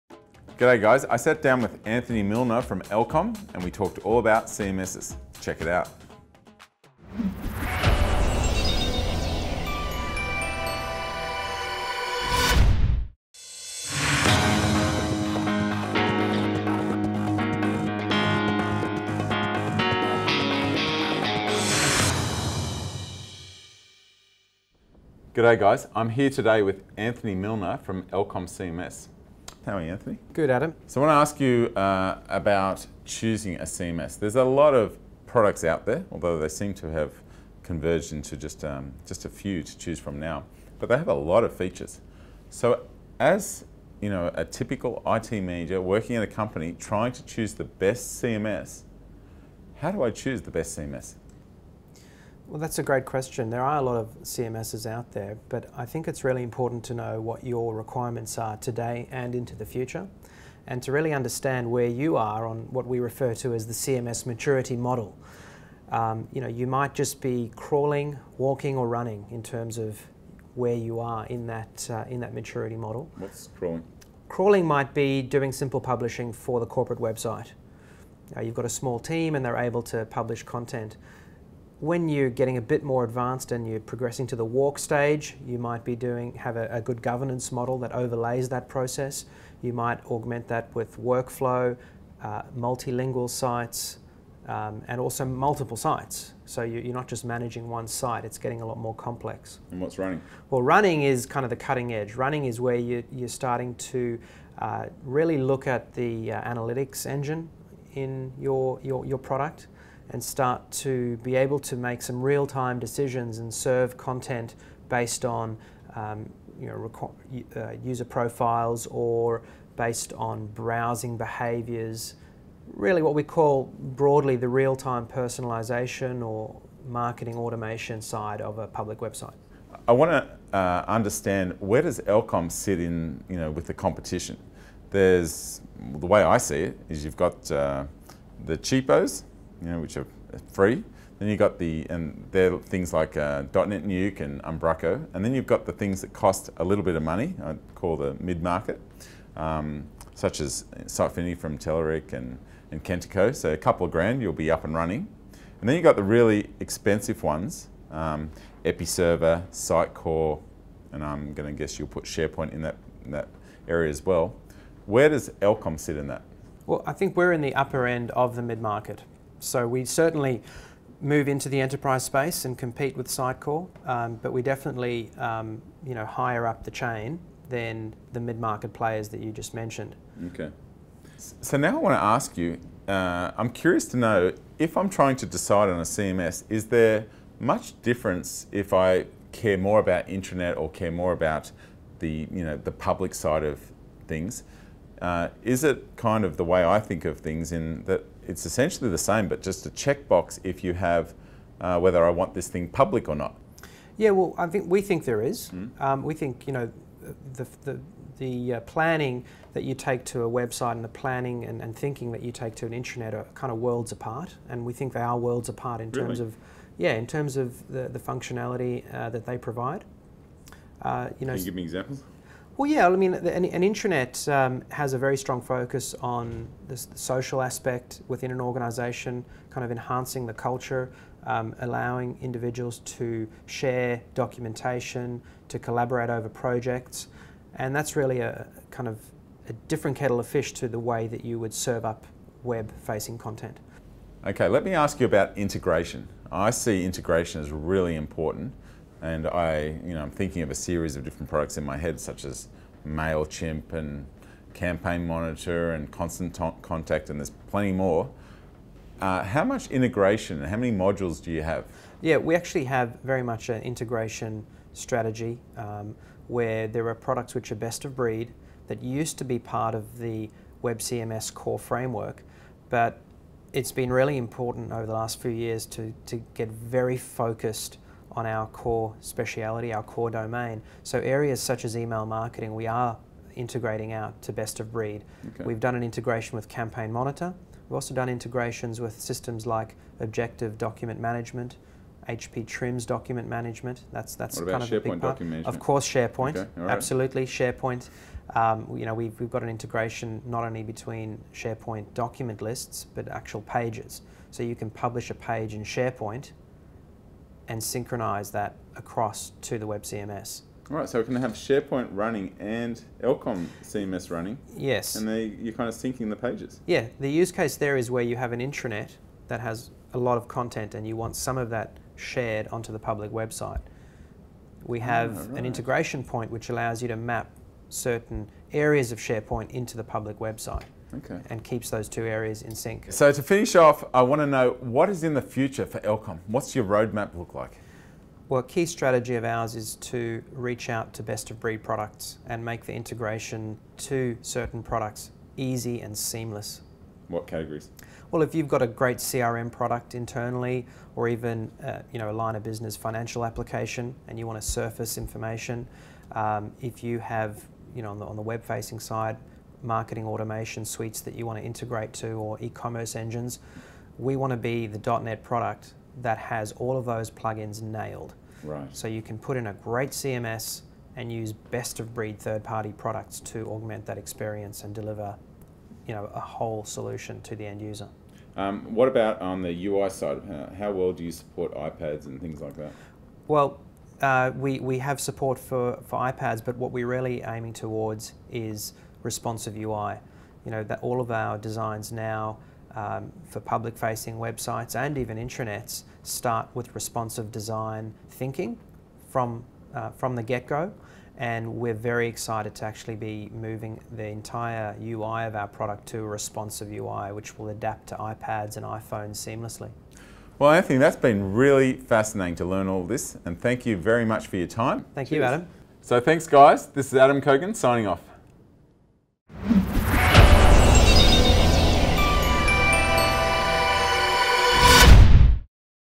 Content Management Systems with Anthony Milner
Adam Cogan sits down with Elcom's Anthony Milner to discuss the Elcom CMS package and the pros and cons you should look for in a CMS.